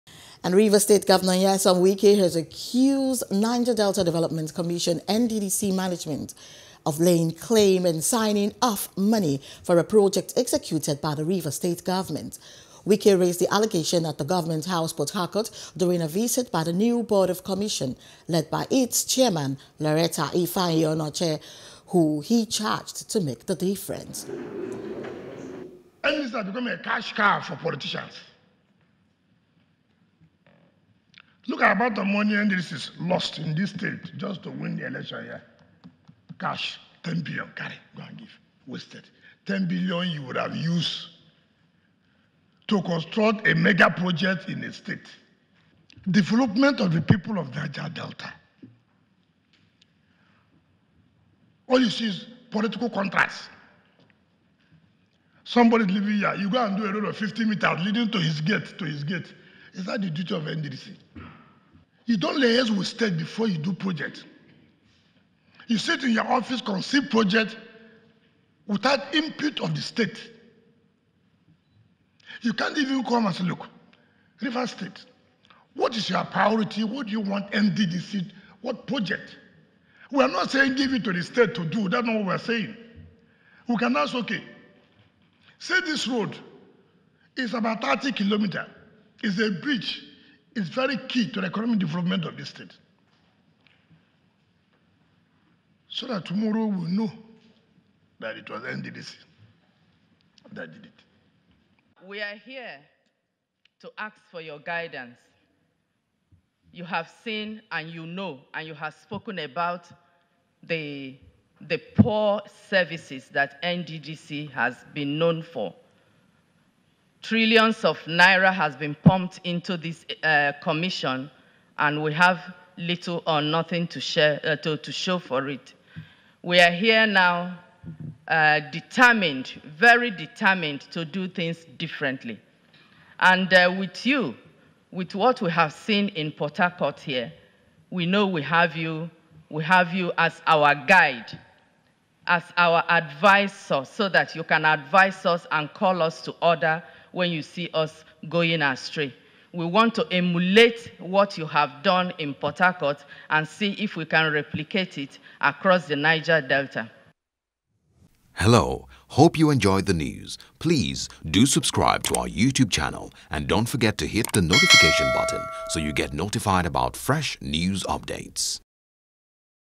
NDDC: Wike Accuses Commission Of Claiming Money For Construction By Rivers Govt
Rivers State GOVERNOR Nyesom Wike has accused Niger Delta Development Commission (NDDC) management of laying claim and signing off money for a project executed by the Rivers State Government.